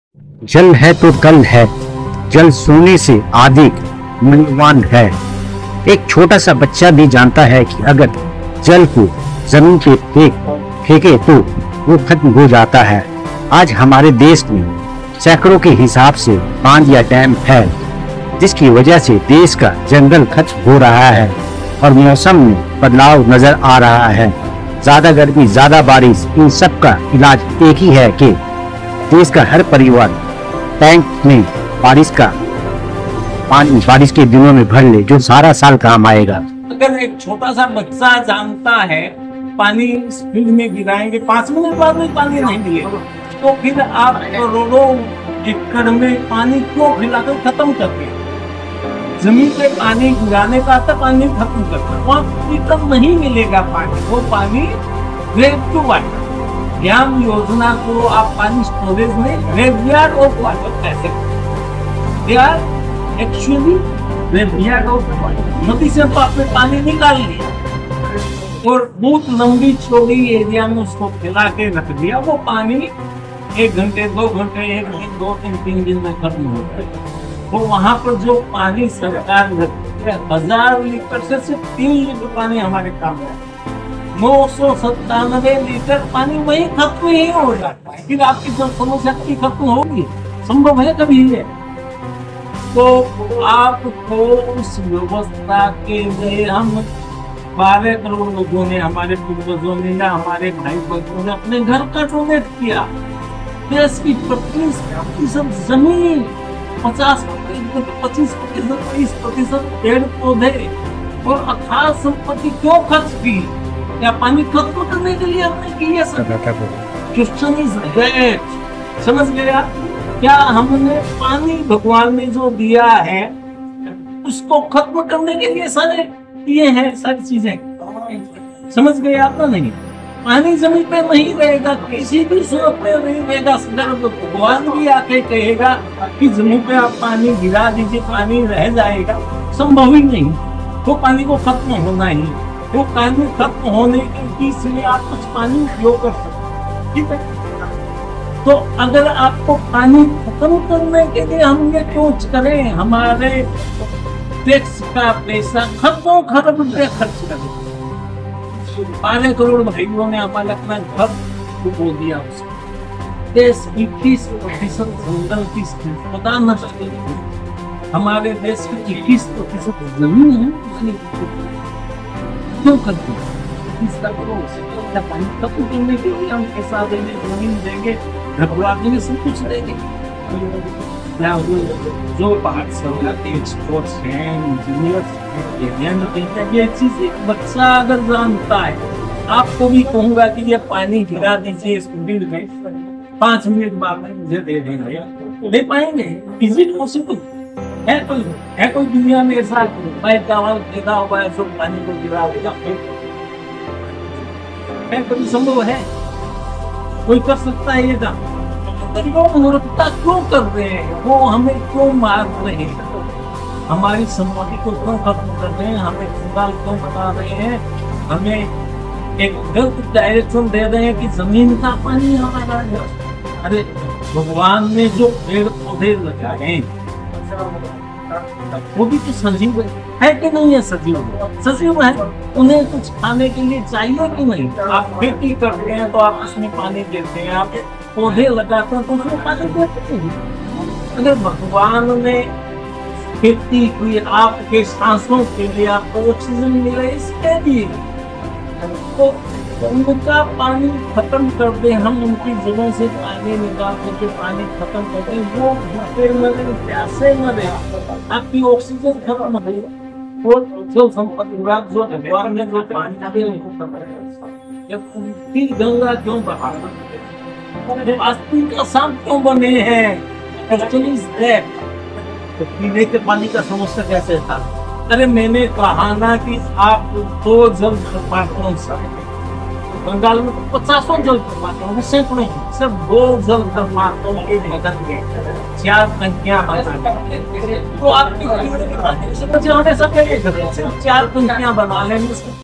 Dr Shyam Sunder Rathi Briefs the Media at Press Club on the Subject of"Importance of Water".
जल है तू कल है, जल सुने से अधिक मूल्यवान है एक छोटा सा बच्चा भी जनता है की अगर जल को जमीन पे फेंके तू वह खत्म हो जाता है आज हमारे देश मै सेंकरों के हिसाब से बांध ya damb है जिसकी वजह से देश का जंगल ख़त्म हो रहा है और मौसम मई बदलाव नज़र आ रहा है ज़्यादा गर्मी ज्यादि बारिश इन सब का इलाज एक ही है के देश का हर परिवार टैंक मई बारिश के दिनों पानी भर ले जो सारा साल काम आएगा